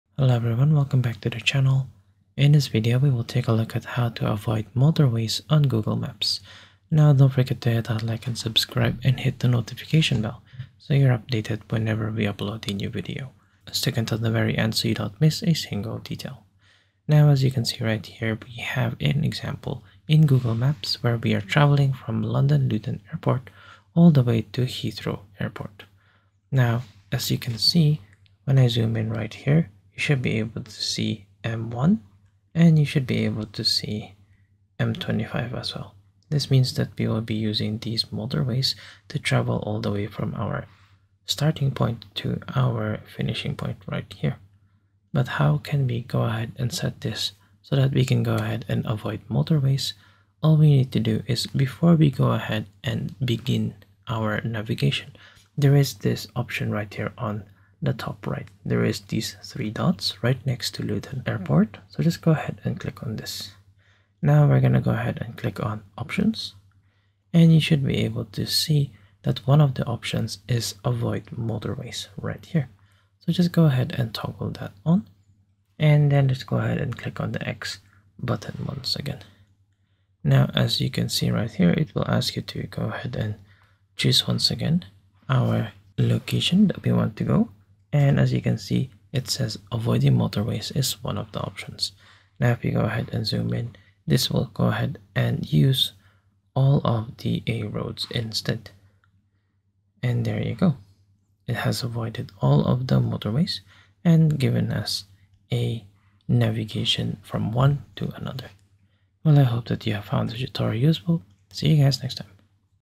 How To Avoid Motorways on Google Maps (Easy Method)
How to avoid motorways in google maps.

In this video I'll show you how to avoid highways google maps. The method is very simple and clearly described in the video. Follow all of the steps & avoid motor or high ways on google maps.